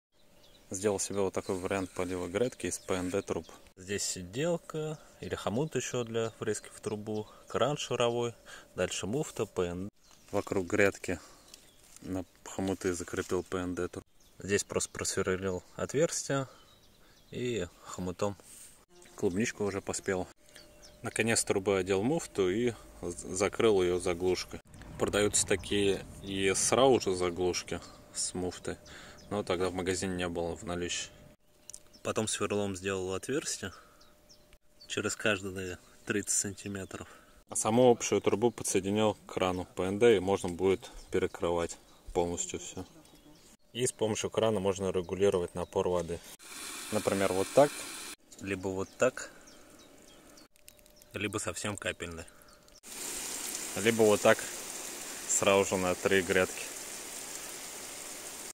Как сделать удобный полив грядки из ПНД труб ?)
Сделал регулируемый полив грядки из пнд трубы, шарового крана, муфт пнт, заглушки. Показываю как он работает и как закрепил трубу, а также чем сверлил отверстия.